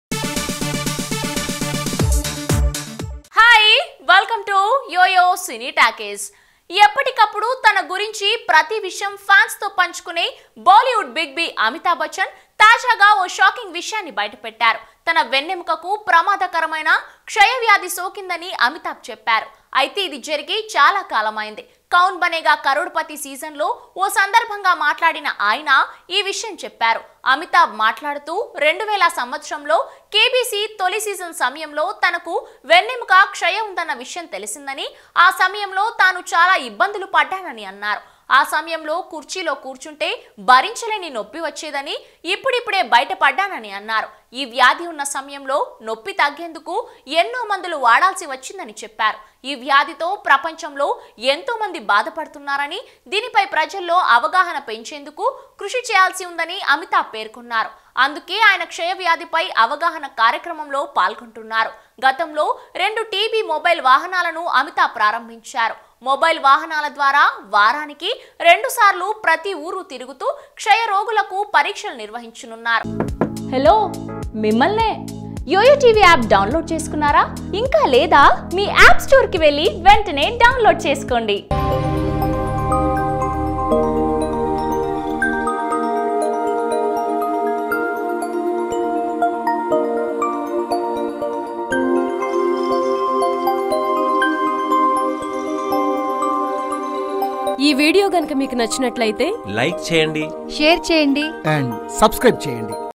Amitabh Bachchan Revealed Shocking Facts About His Health | Tollywood | YOYO Cine Talkies
Amitabh Bachchan Revealed Shocking Facts About His Health

Latest theatrical trailers of Movies like sye raa Narasimha reddy, ntr biopic, 2.0, taxiwala, rrr, maharshi,